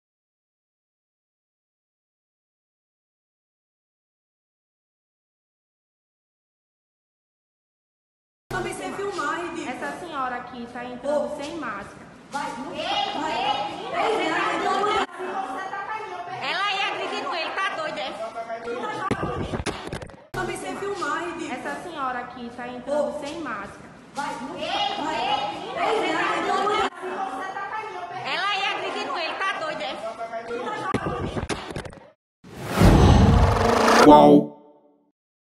Vídeo flagra momento em que mulher sem máscara agride enfermeiras em posto de vacinação em Recife
Uma mulher que compareceu sem máscara ao posto de vacinação no Ginásio Geraldão, em Recife, causou confusão e agrediu uma enfermeira e uma técnica de enfermagem que registraram tudo em vídeo.